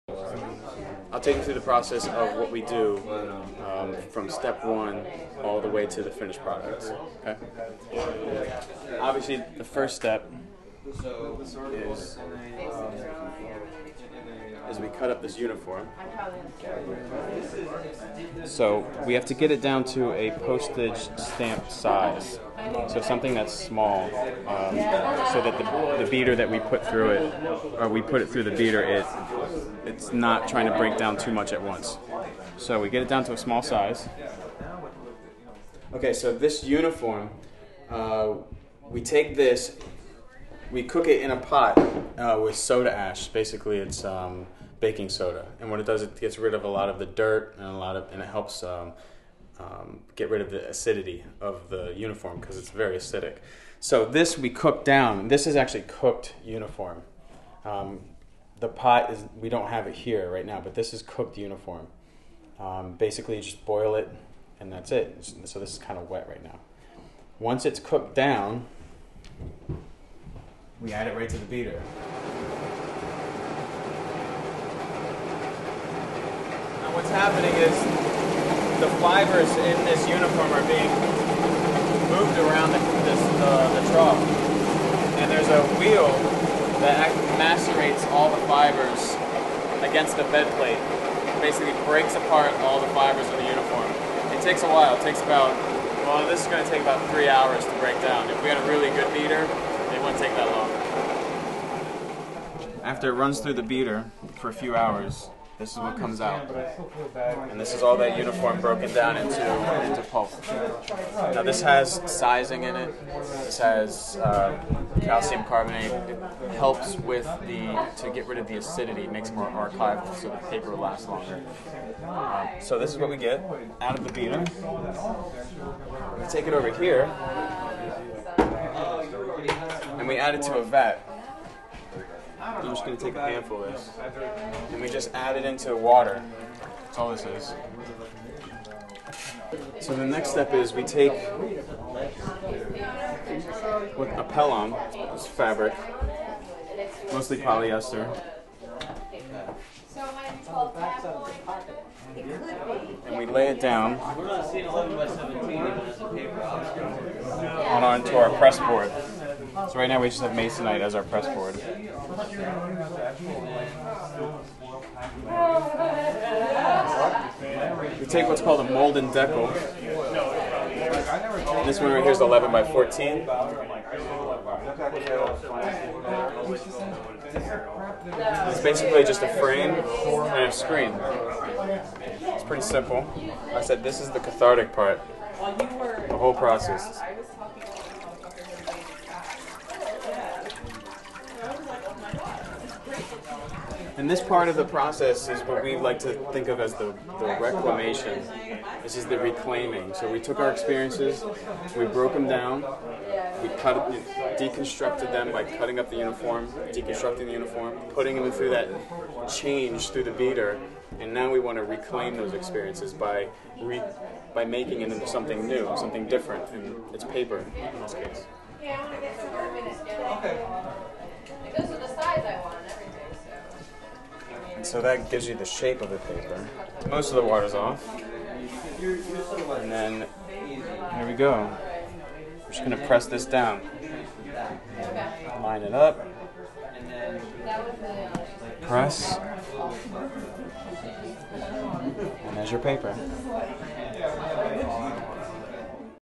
Making Your Own 'Combat Paper': A Step-by-Step Tutorial
Davide Keefe of the Combat Paper Project talks us through the cathartic paper-making process used to create each leaf of paper from veterans' military uniform. The workshop assists veterans in sharing their personal military experience through art.